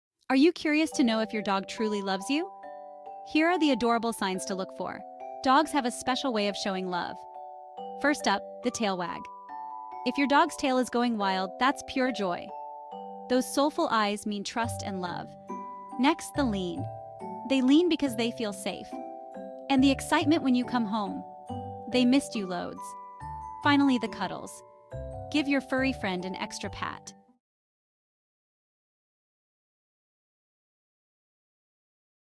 How to Know Your Dog Loves You 🐶❤️
Discover the heartwarming ways dogs show love in our latest video! From the joyful wagging of their tails to the soulful eye contact that speaks volumes, learn how to recognize the unique behaviors that signify your dog's deep affection. Watch as we explore how leaning against you and their sheer excitement when you return home are clear indicators of their love and trust. Don't miss these endearing moments that highlight the special bond between you and your furry friend.

See Less

OUTLINE:
00:00:00